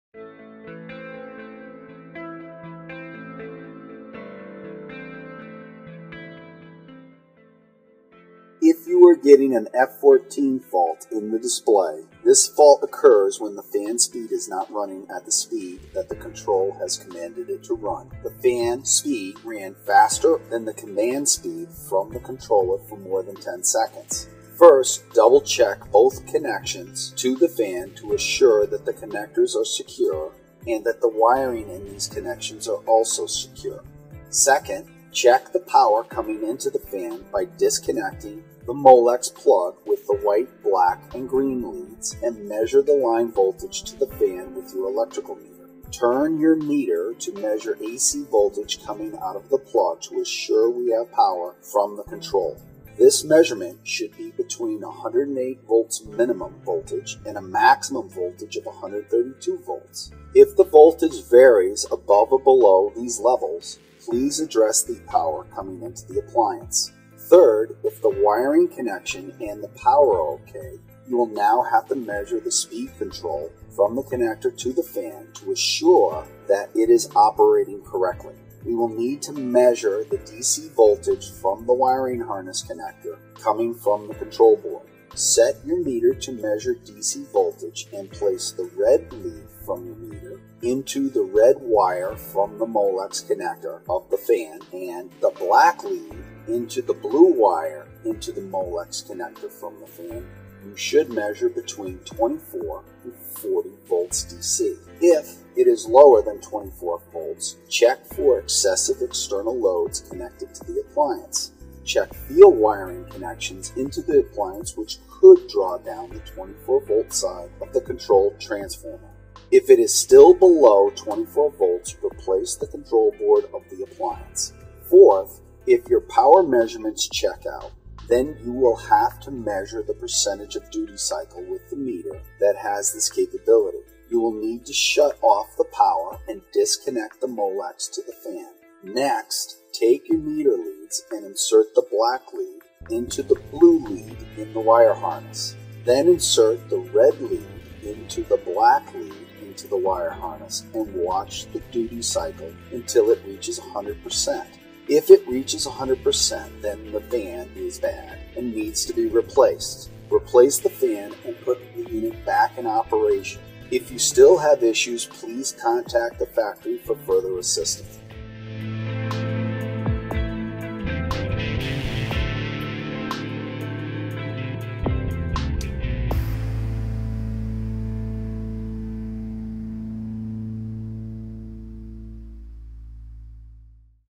F14 Fault Code -
HTP: F14 Fault Code: Combustion fan speed high -- The heater will lock out if it senses that the fan speed is less than 130% of expected rate for more than 60 seconds

1. Check the combustion air fan wiring.
2. Replace the combustion air fan.
3. Replace the control board.